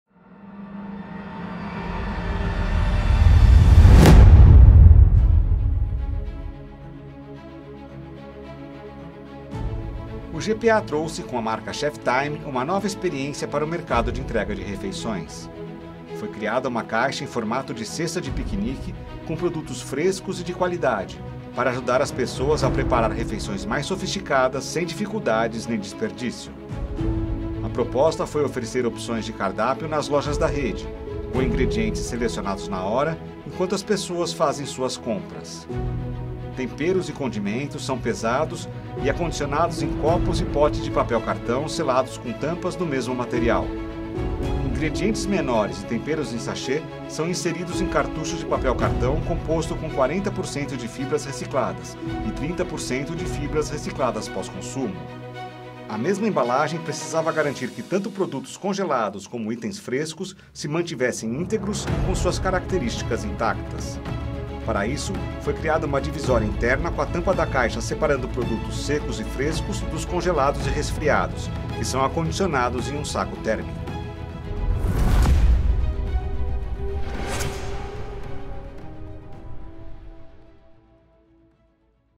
GPA Cheftime - PRÊMIO GRANDES CASES DE EMBALAGEM 2022
GPA, com a marca Cheftime, criou uma caixa em formato de cesta de piquenique com produtos frescos